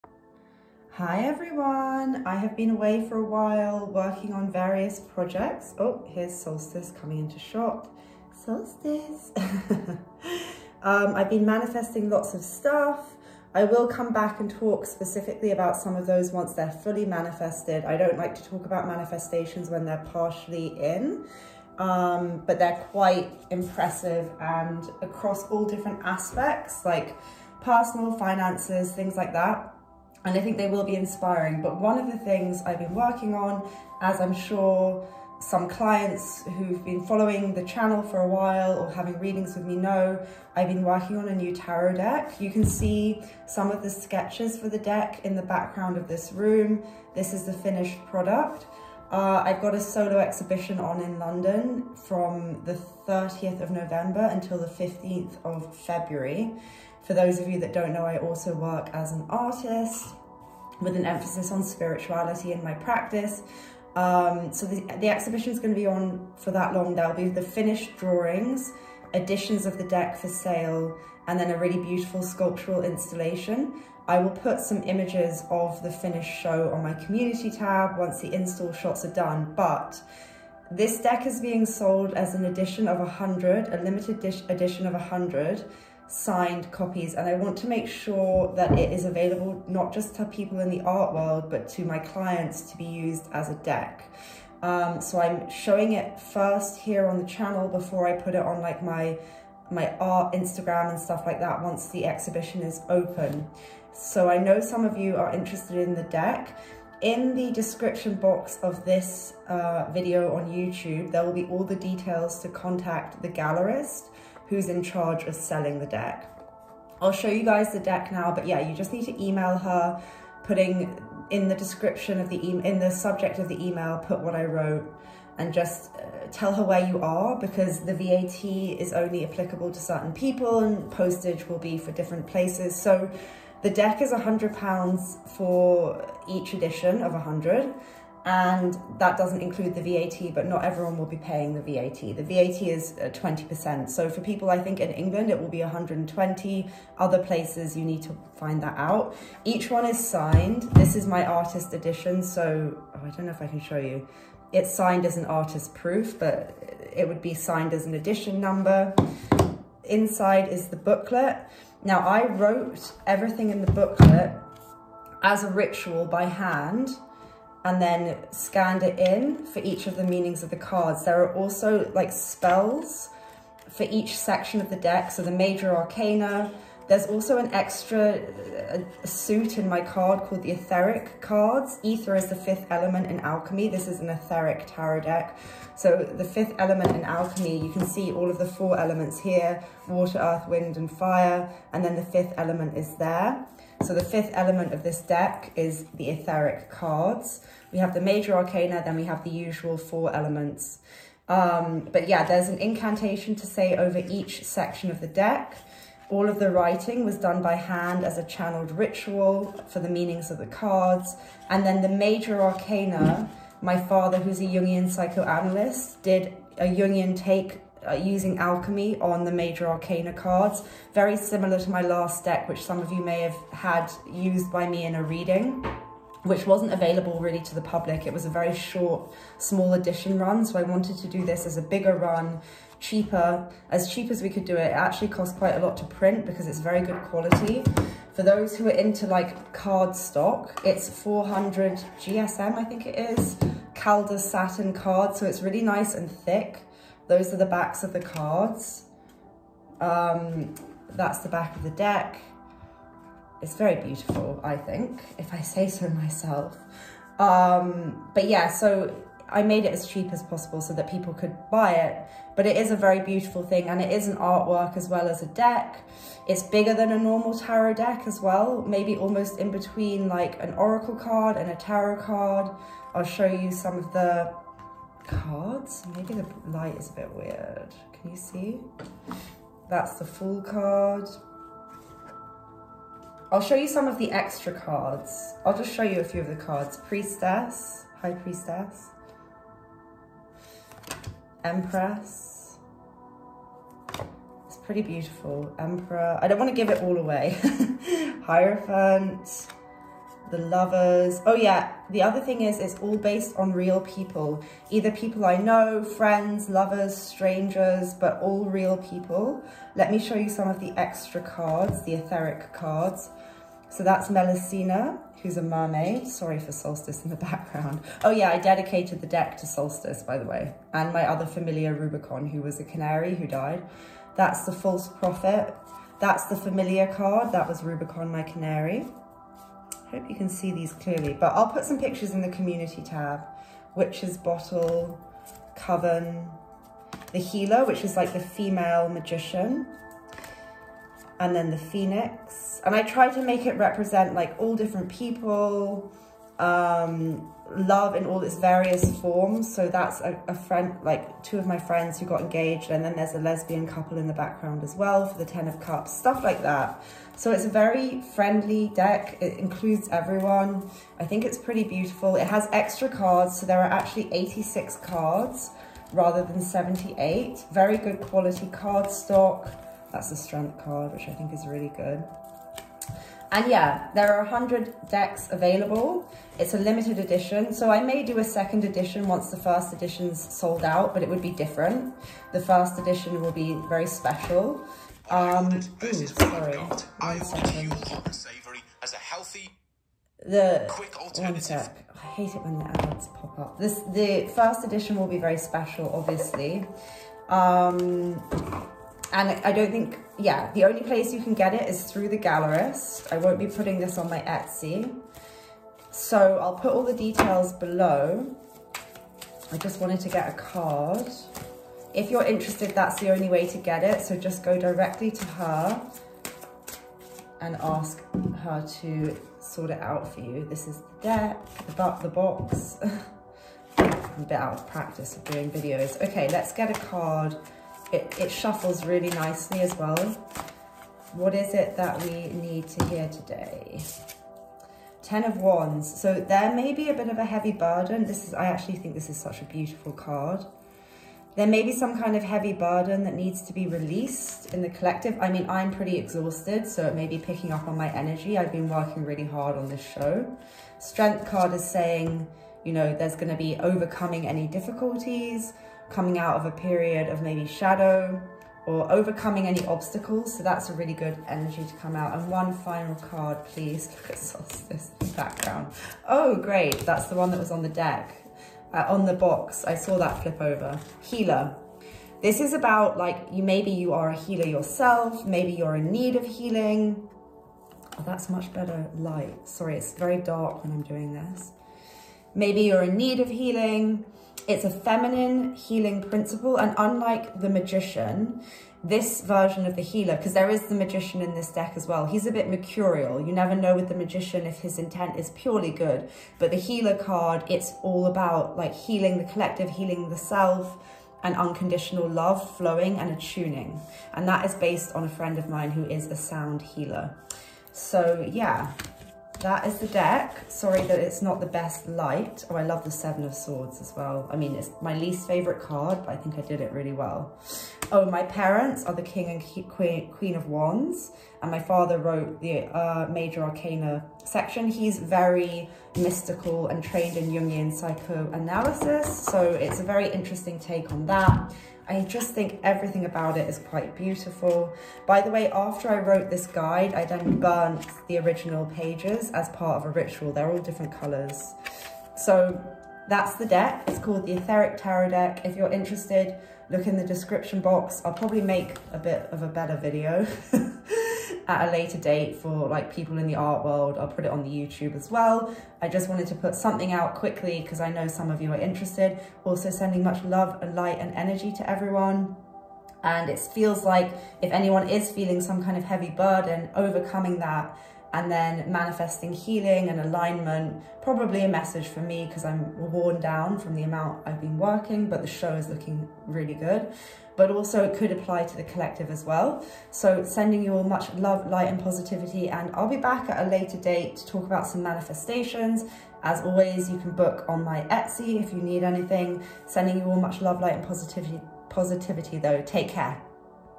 The AETHERIC TAROT - New Tarot Deck, Designed by ME, AVAILABLE to BUY NOW ! 🔮
Let Keiko know you’d like to buy an edition and where you are - she will sort out the payment and postage.

The Aetheric Tarot is a signed limited edition of 100 decks. Each deck will be £100 plus VAT (if applicable) and postage.

If you live in London you can pick it up in person from my exhibition until 15th February 2024.

Much love and light to all ✨✨✨